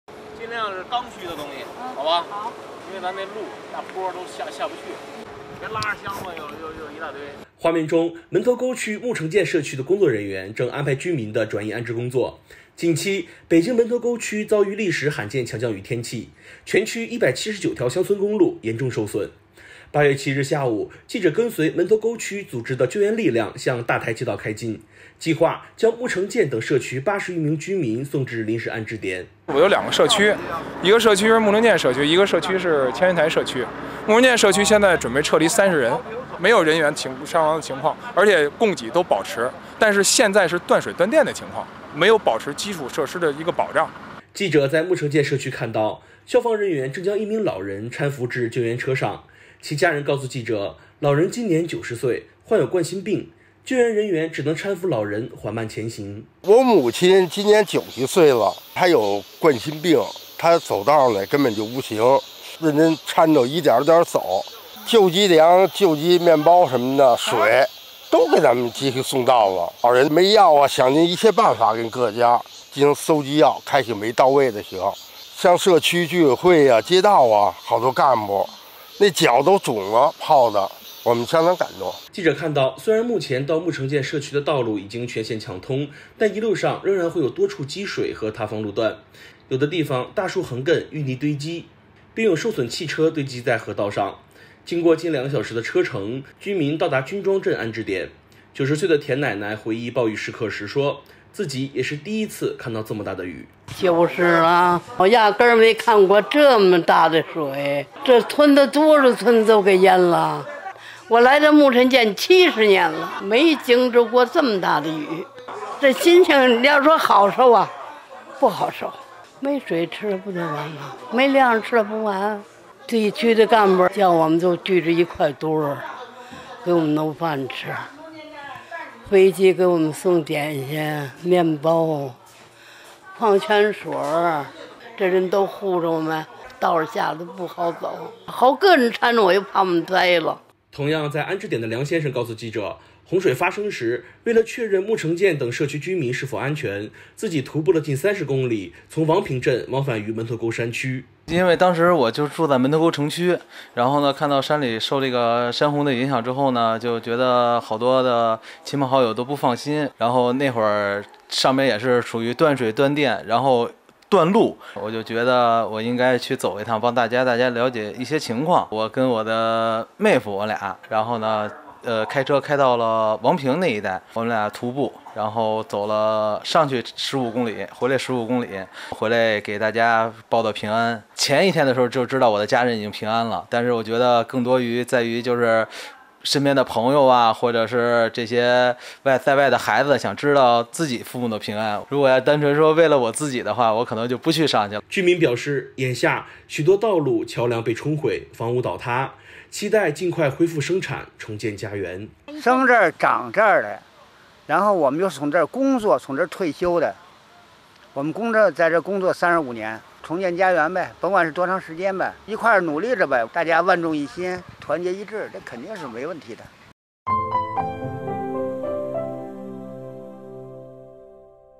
现场直击：北京门头沟木城涧等社区80余人被安全转移
近日，门头沟区木城涧社区的工作人员正安排居民的转移安置工作。近期，北京门头沟区遭遇历史罕见强降雨天气，全区179条乡村公路严重受损。8月7日下午，记者跟随门头沟区组织的救援力量向大台街道开进，计划将木城涧等社区80余名居民送至临时安置点。（迟瀚宇）